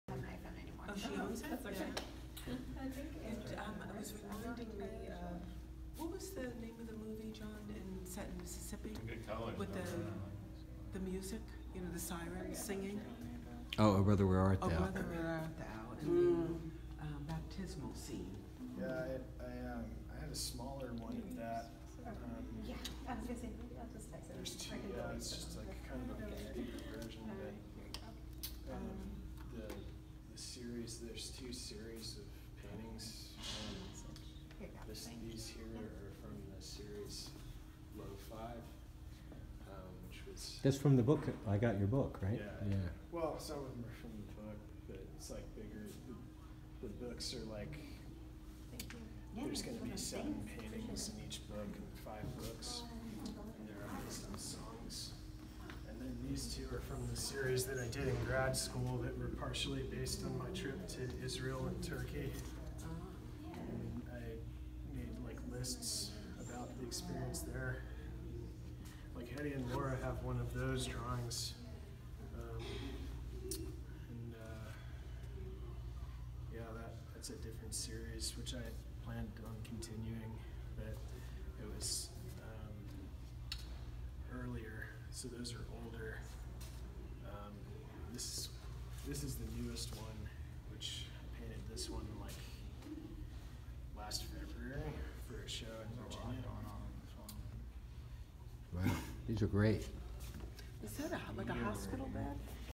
Walter Sutin art from San Diego Dada, Oceanside museum
2019 at the surreal San Diego visit with Walter Sutin, Phyllis Messinger, Anna , Daniel,, and John Straus